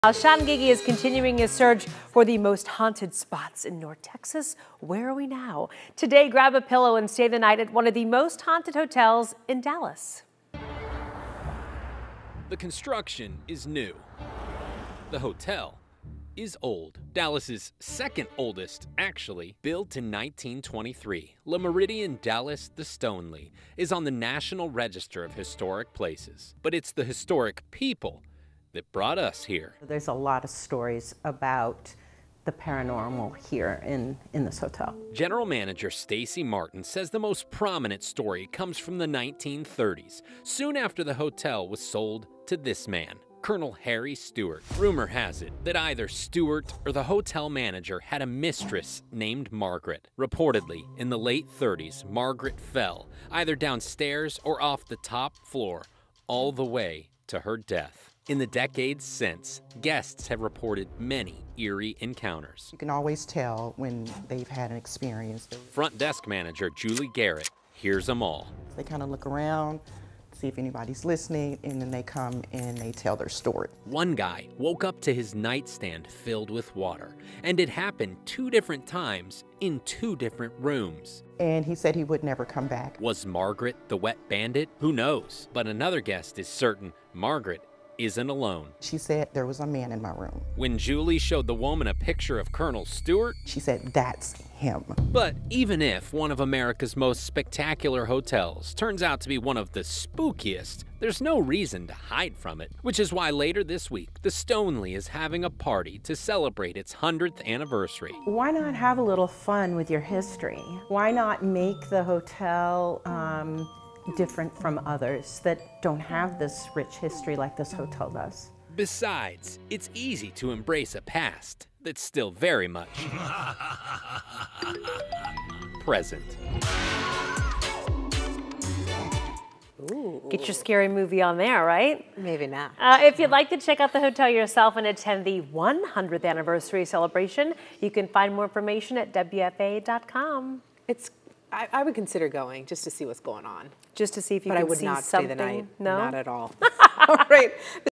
One of Dallas’ oldest hotels is also one of its most haunted
Guests and employees at Le Meridien Dallas, The Stoneleigh say they’ve experienced unexplained happenings for nearly a century.